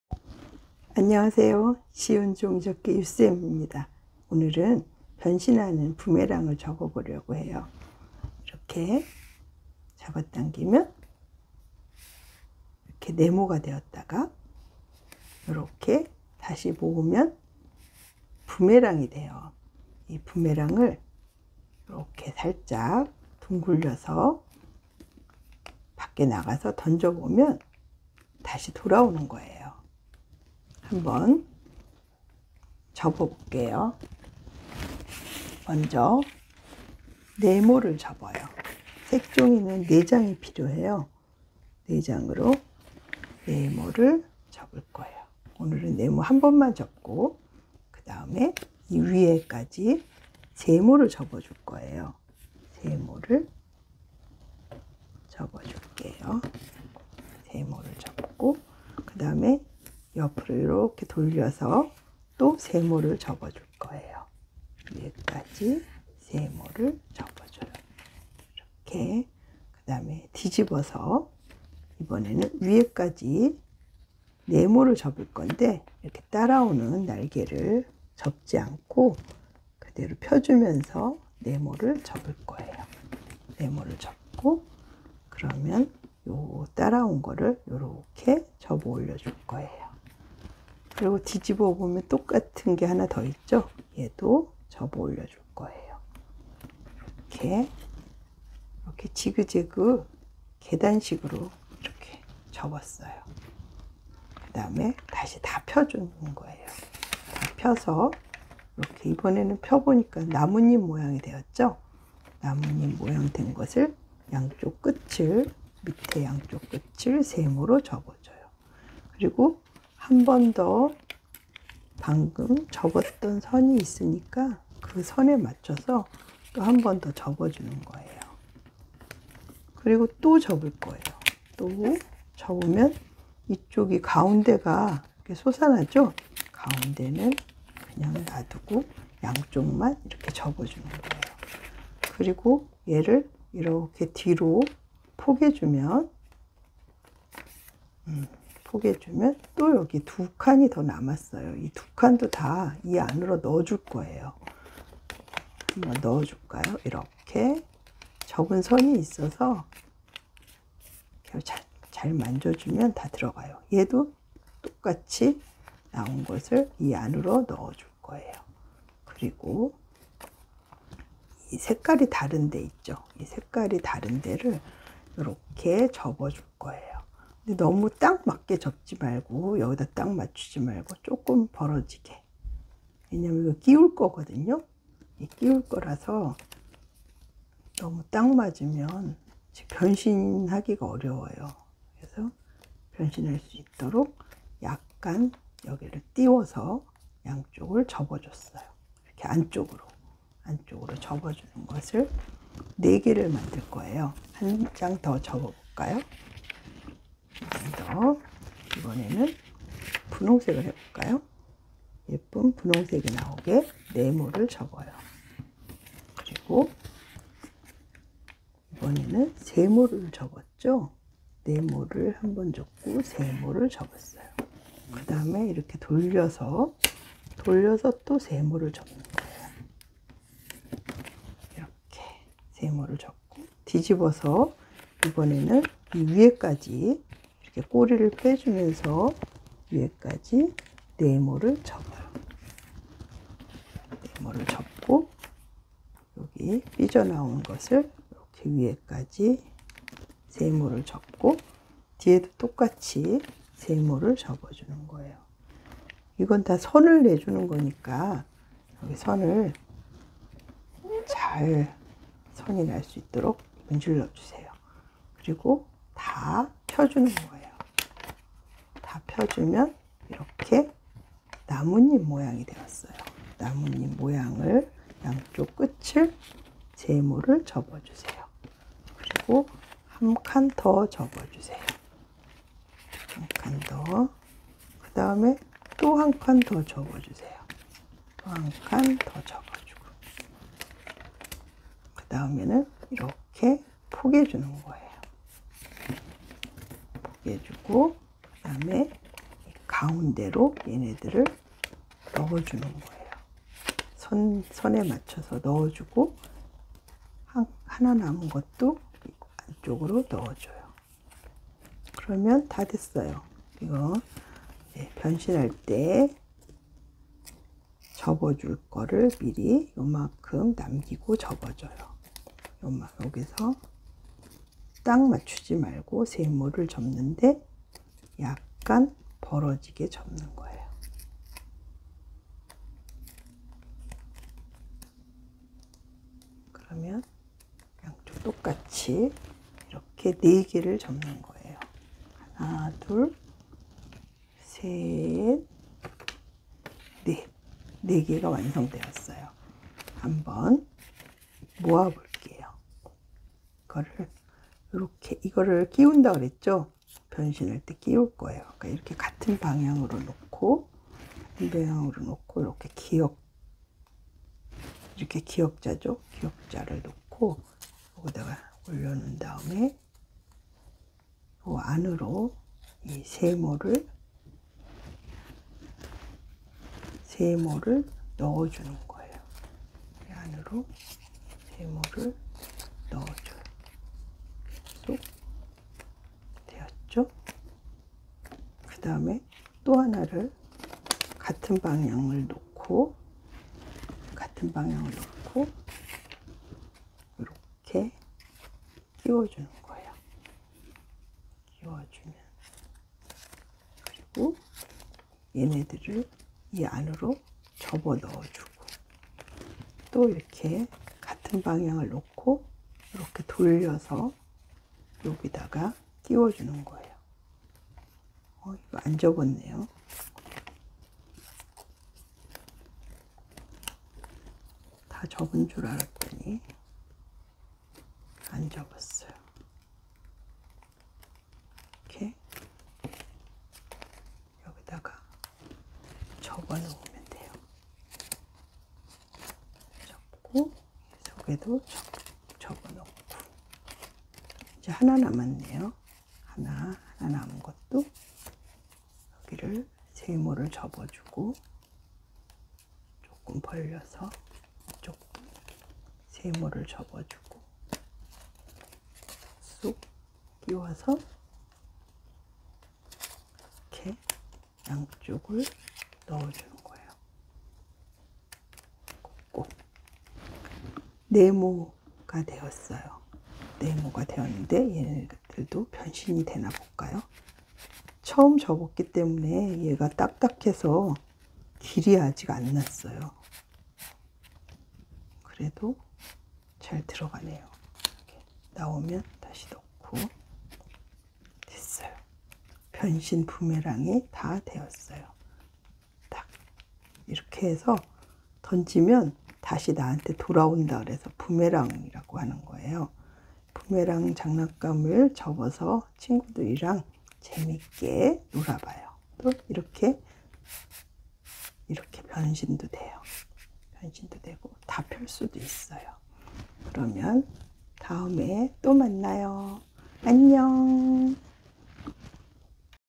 변신부메랑 종이접기
15 ×15cm 양면색종이 4장으로 접었어요~~
다접은후 동그랗게 말아보세요~
부메랑이 돌아올거예요 ~